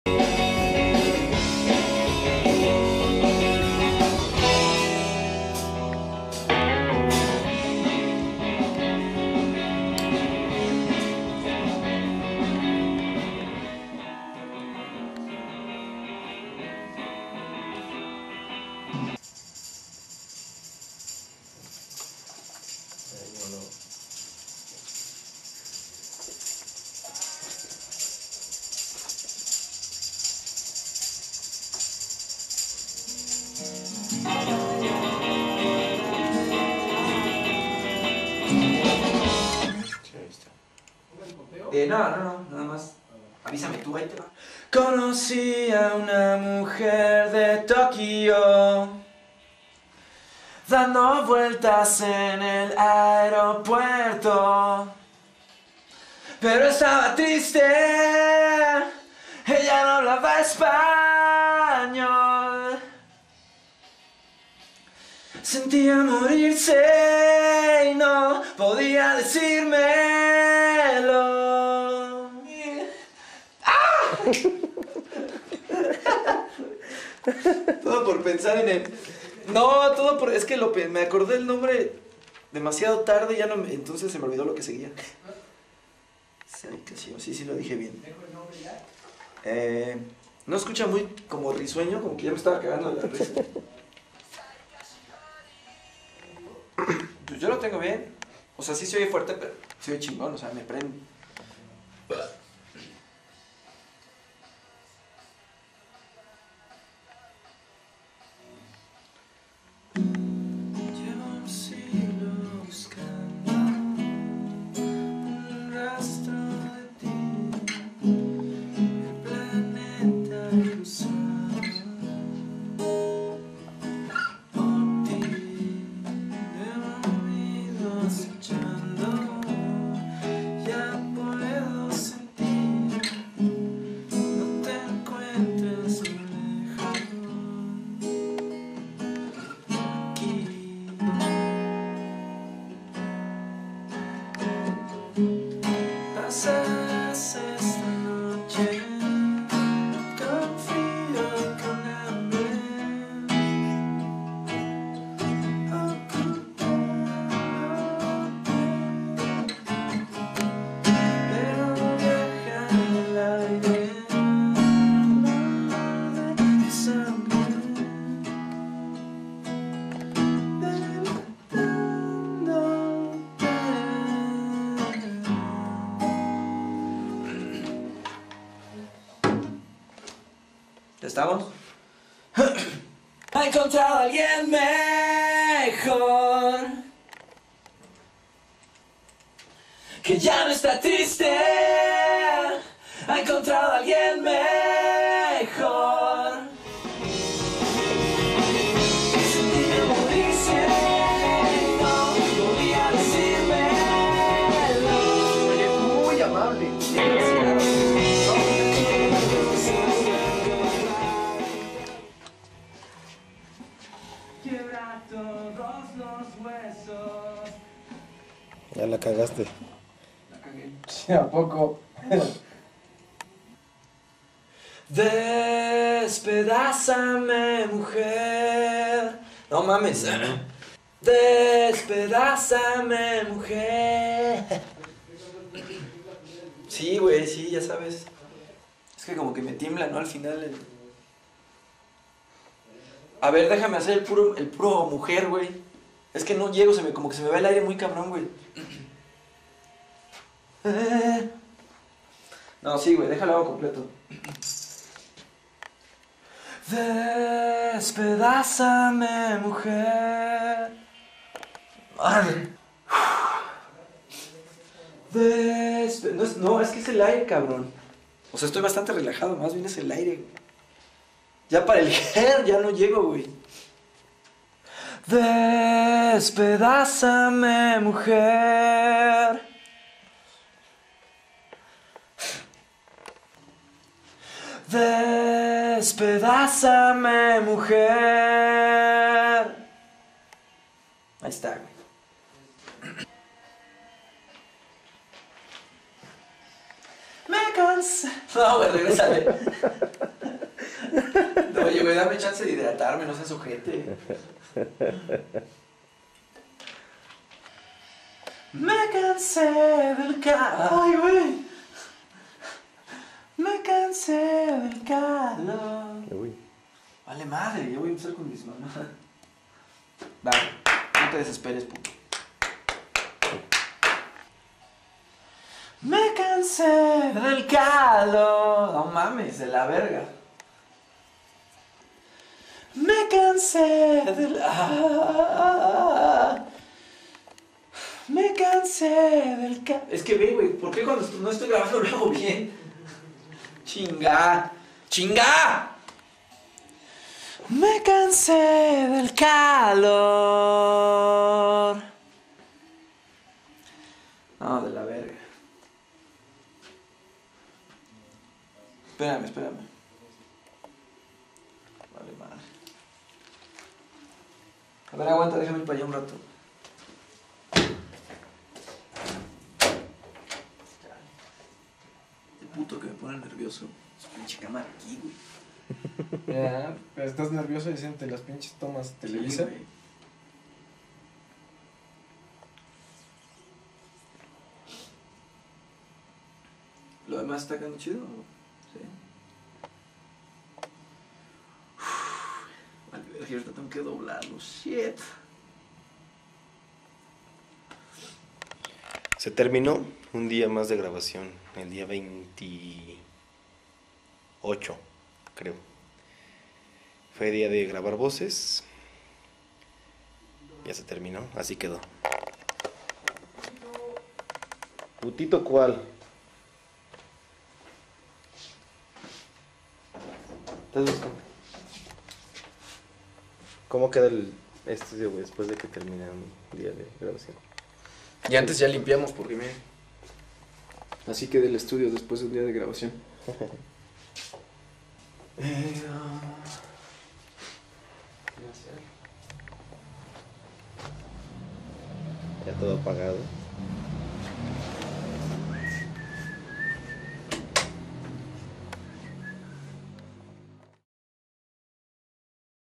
Eufemia - Grabación 3er Disco Día 28 / Eufemi-Cam
Diario de la grabación del tercer disco de Eufemia. 17 de diciembre de 2008.